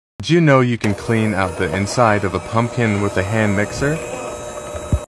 The best way to clean out the insides of a pumpkin 🎃
what are you carving into your pumpkins this halloween? 🎃

— Recommendations are independently chosen by Reviewed’s editors. Purchases you make through our links may earn us a commission.